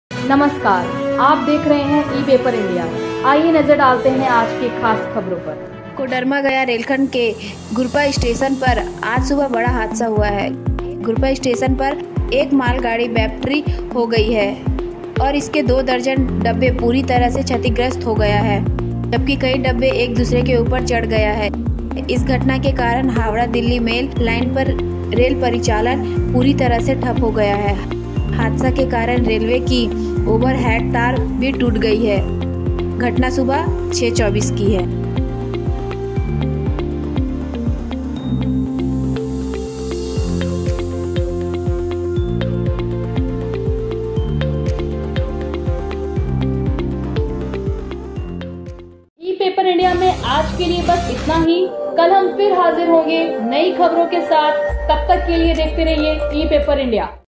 बिहार में बड़ा रेल हादसा- गुरपा स्टेशन पर ट्रेन के डिब्बे पटरी से उतरे | BREAKING NEWS
video description :

कोडरमा गया रेलखंड के गुरपा स्टेशन पर आज सुबह बड़ा हादसा हुआ है। गुरपा स्टेशन पर एक  मालगाड़ी बेपटरी हो गई है और इसके दो दर्जन डब्बे पूरी तरह से क्षतिग्रस्त हो गए हैं, जबकि कई डब्बे एक दूसरे के ऊपर चढ़ गए हैं। इस घटना के कारण हावड़ा दिल्ली मेल लाइन पर रेल परिचालन पूरी तरह से ठप हो गया है। हादसे के कारण रेलवे की ओवरहेड तार भी टूट गई है। घटना सुबह 6:24 मिनट की है।